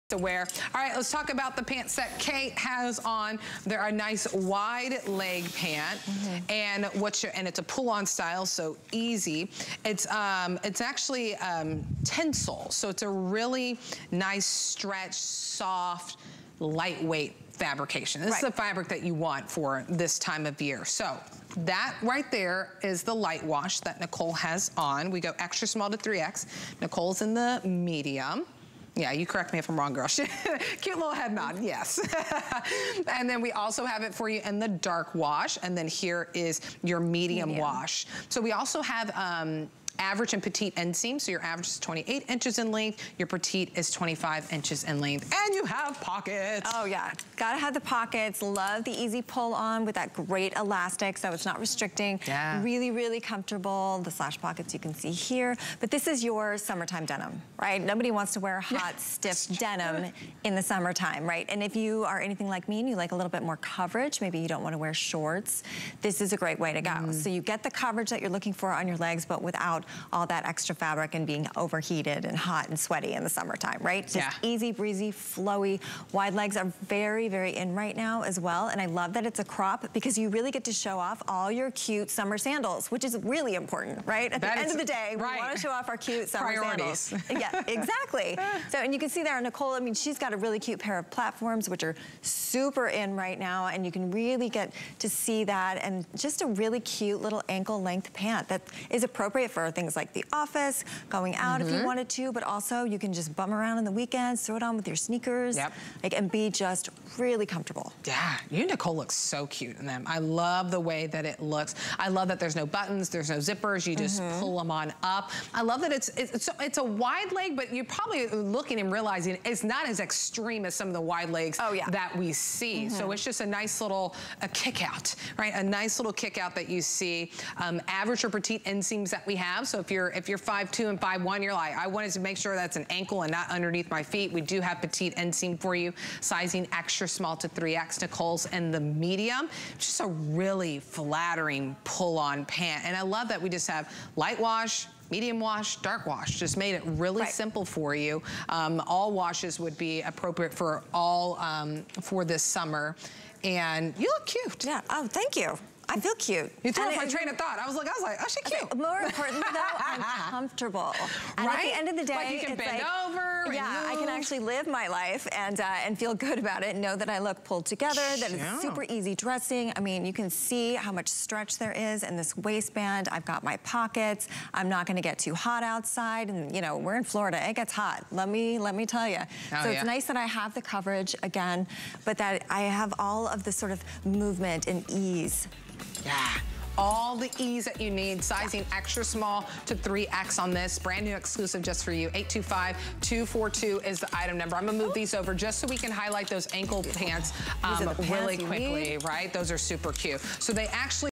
IMAN Global Chic Chambray Wide Leg Pullon Pant
For a staple piece that's sure to last for seasons to come, this wideleg chambray pant provides yearround style while taking your wardrobe for miles.